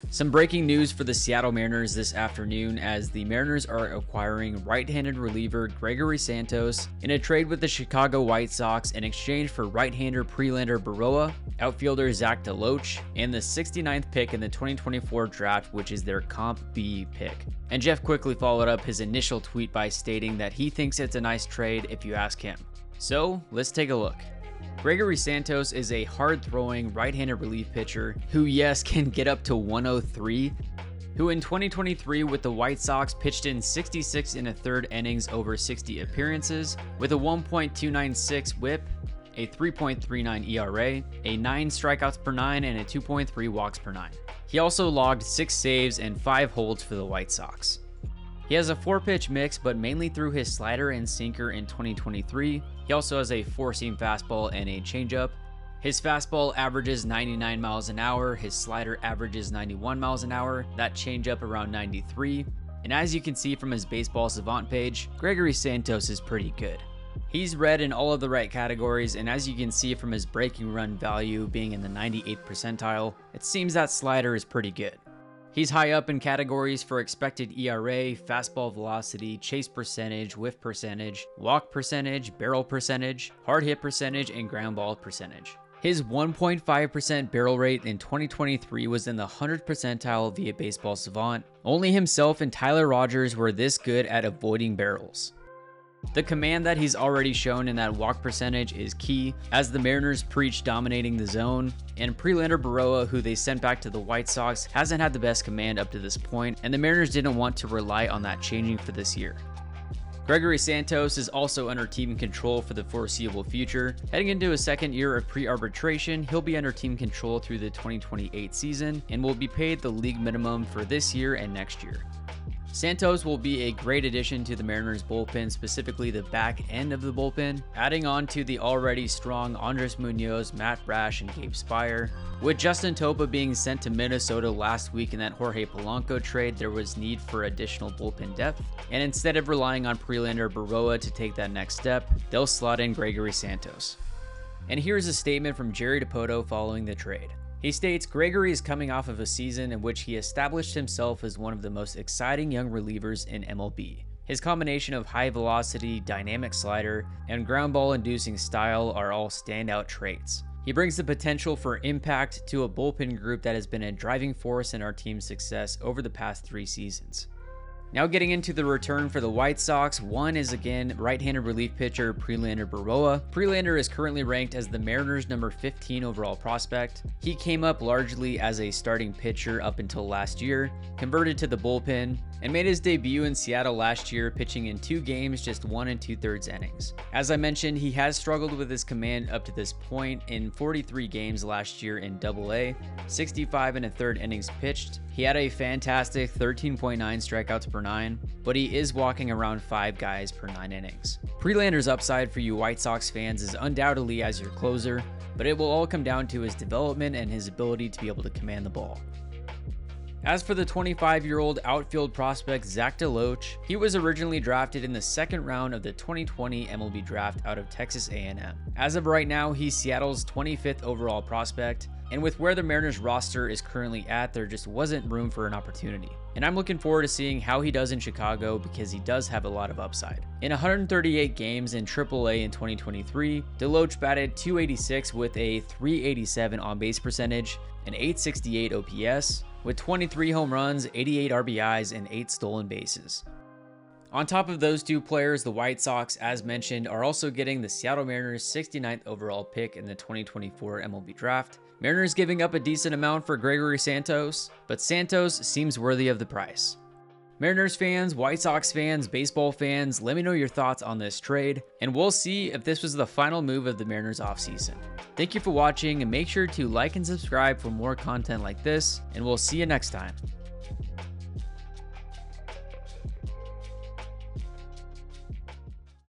Seattle Mariners Acquire RHP Gregory Santos from the White Sox!
Today it was announced that the Seattle Mariners acquired hard throwing relief pitcher Gregory Santos from the Chicago White Sox in exchange for RHP prospect Prelander Berroa, OF prospect Zach DeLoach, and the 69th overall pick in the 2024 MLB Draft.

Connor Webb Mortgage Loan Advisor, NMLS #1529504

Advantage Mortgage, Inc. NMLS #1770599